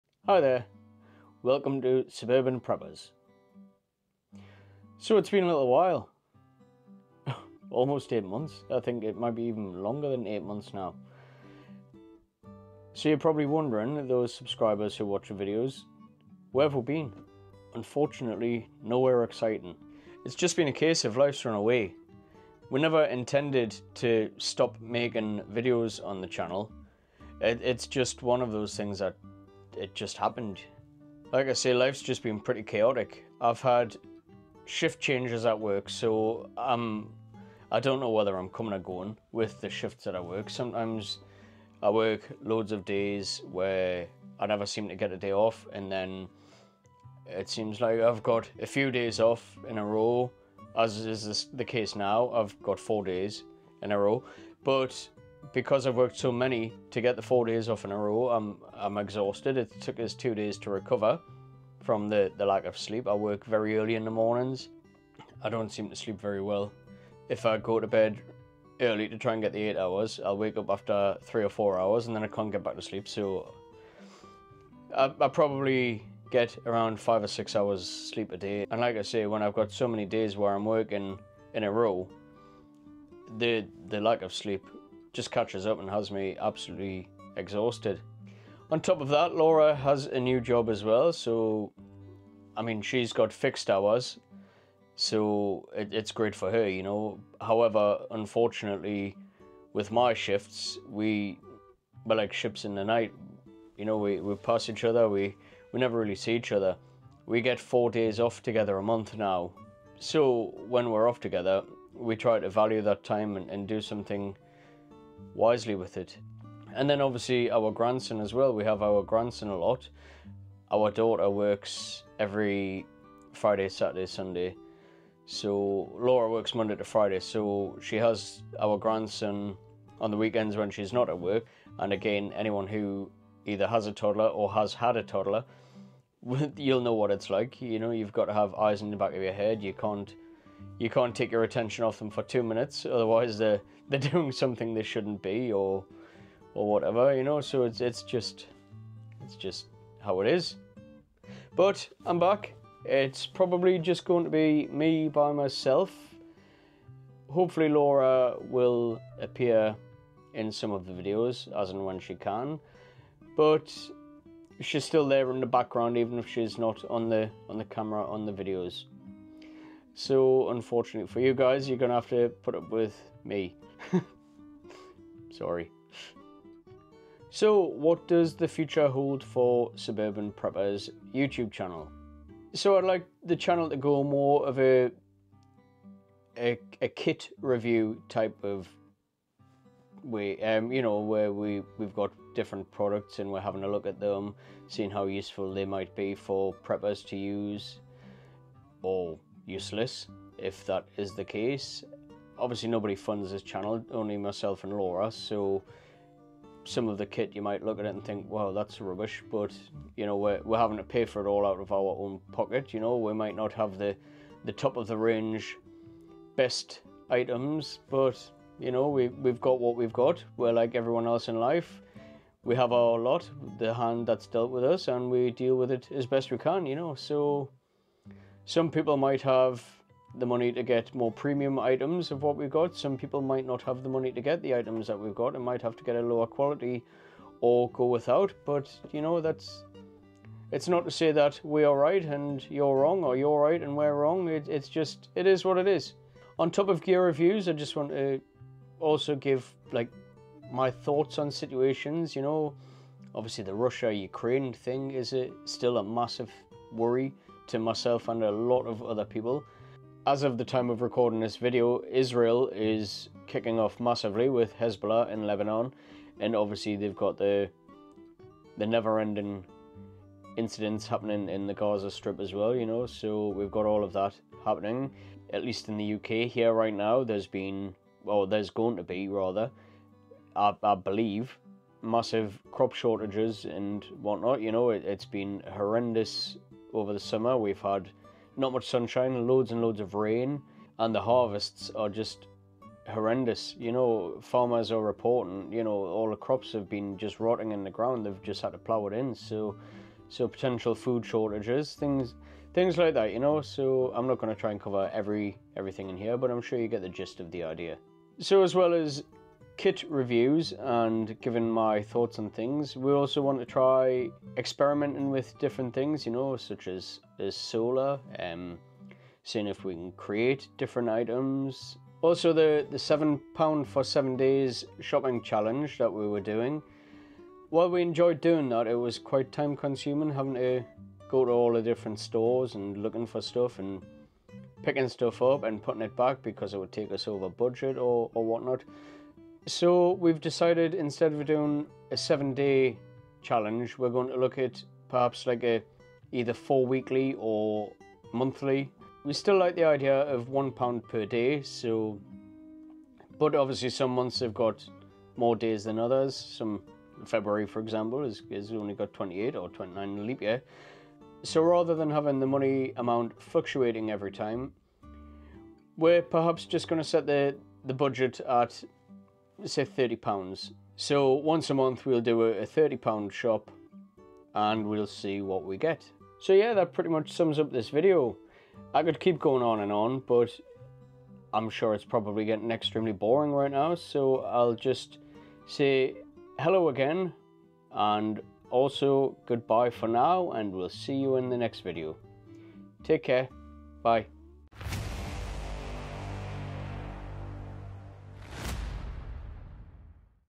UK Preppers: The Update You’ve All Been Waiting For
Hello. Its been a little while since we uploaded a video. Life has been a little bit crazy for us for the last 8 months or so, & we kind of just never got around to doing more videos on here. It wasn't done on purpose, it just kind of happened.

But we're back. We'll have weekly videos again from now on. These could be anything from product reviews, tips & suggestion, right through to our thoughts on any subject or situation.

We want to finish off by saying thank you to the subscribers who stuck with us over the month. And also welcome to the subscribers we have gained while we took our foot off the pedal.

We hope you enjoy this video & all of our other videos too.

Stay safe. Stay prepared.